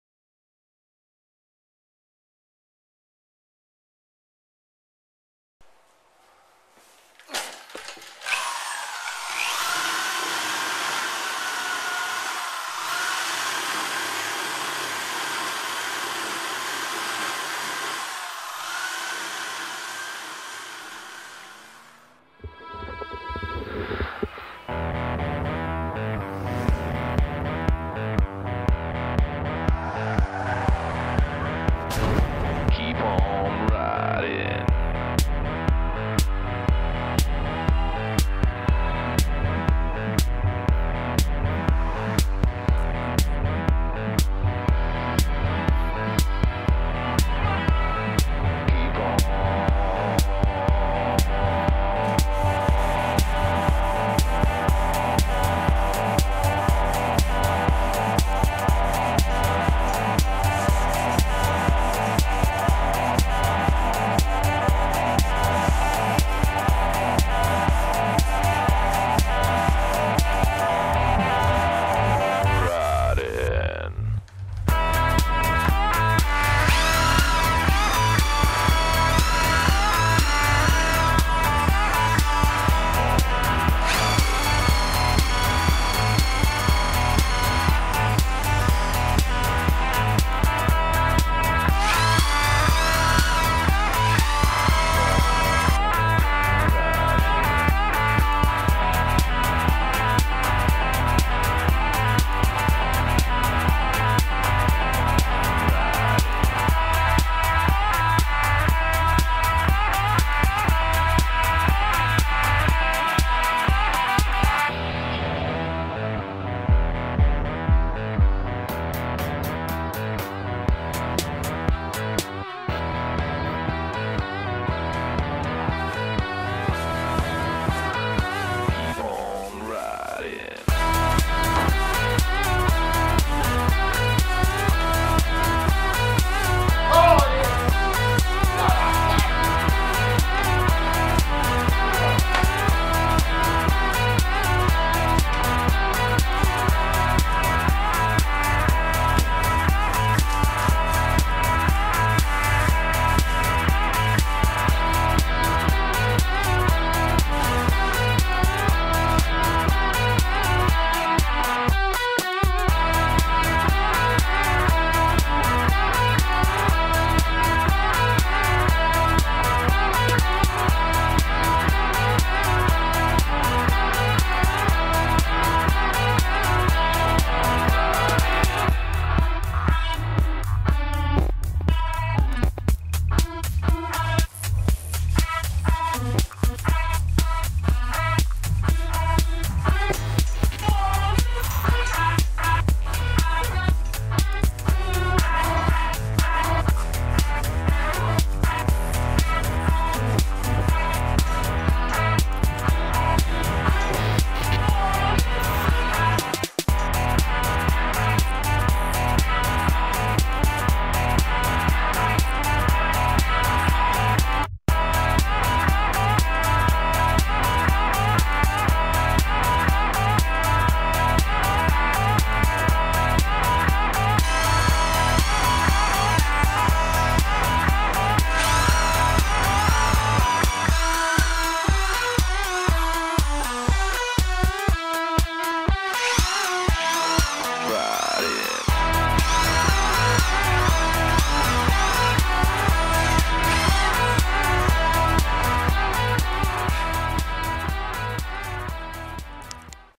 Removing glue, sticker and even silicone
This 3M puck will remove about anything sticky. In this vid I'm prepping a fibreglass pop top from a VW camper van. For sanding I'm using 3M Scotch-Brite # 37447 with water (less dust)